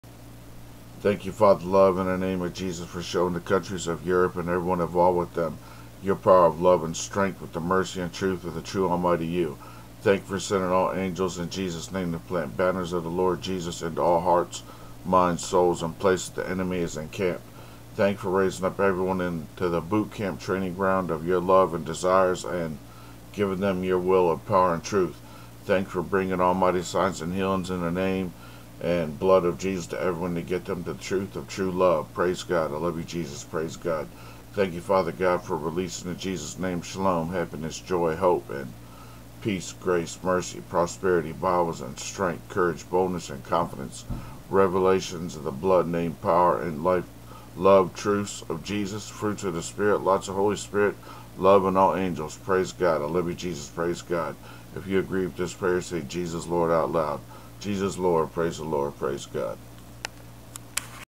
Prayer 180. Europe.
Thank YOU FATHER LOVE,in the name of JESUS for showing the country's of Europe and everyone involved with them YOUR POWER of LOVE and STRENGTH with the MERCY and TRUTH of the TRUE ALMIGHTY YOU !!! Thank YOU for sending all angels,in JESUS name,to plant banners of the LORD JESUS,into all hearts,minds,souls and places the enemy is encamped !!! Thank YOU for raising up everyone into the boot-camp-training-ground of YOUR LOVE and DESIRES and giving them YOUR WILL of POWER  and TRUTH !!! Thank YOU for bringing ALMIGHTY signs and healing's in the name and BLOOD of JESUS to everyone to get them to the TRUTH of TRUE LOVE !!! Praise GOD !!! I LOVE YOU JESUS !!! Praise GOD !!! Thank YOU FATHER GOD for releasing,in JESUS name,SHALOM,happiness,joy,hope and peace,grace,mercy,prosperity,Bibles and strength,courage,boldness and confidence,Revelations of the BLOOD/NAME/POWER and LOVE/TRUTHS of JESUS,Fruits of the SPIRIT,lots of HOLY SPIRIT,love and all angels !!! Praise GOD !!! I love YOU JESUS !!! Praise GOD !!! If you agree with this prayer say JESUS is LORD out loud !!! JESUS is LORD !!! Praise the LORD !!! Praise GOD !!!